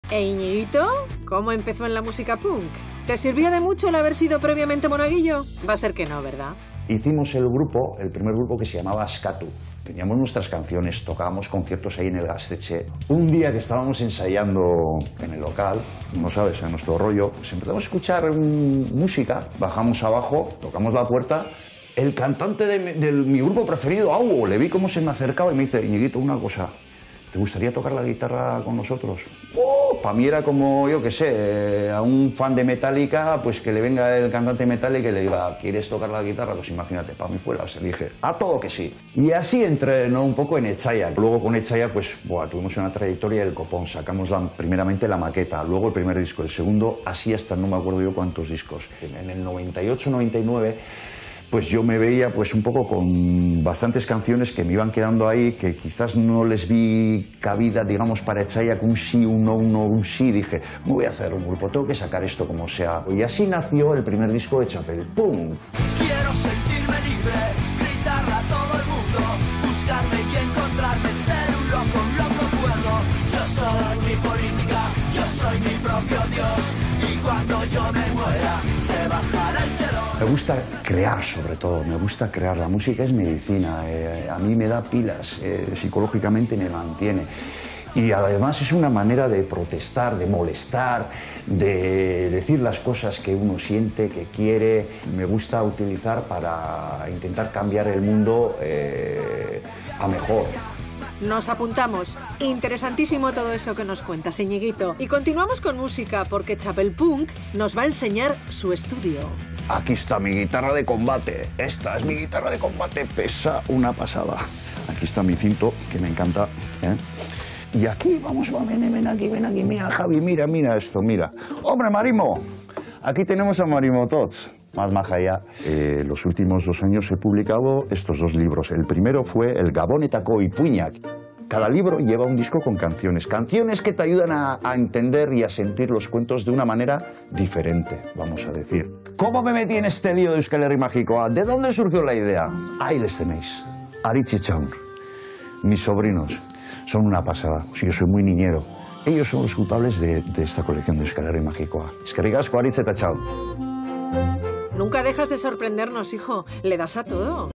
Iñigito lo mismo te saca discos con Etsaiak y Txapelpunk que publica libro-discos para niños
Iñigito Txapelpunk nos hace un resumen de su carrera artística. Comenzó con el grupo Askatu y luego Auo, cantante de Etsaiak, lo fichó como guitarrista para su grupo. Más tarde formó Txapelpunk, y en los últimos años ha sacado dos libro-discos para niños en la colección "E. H. magikoa".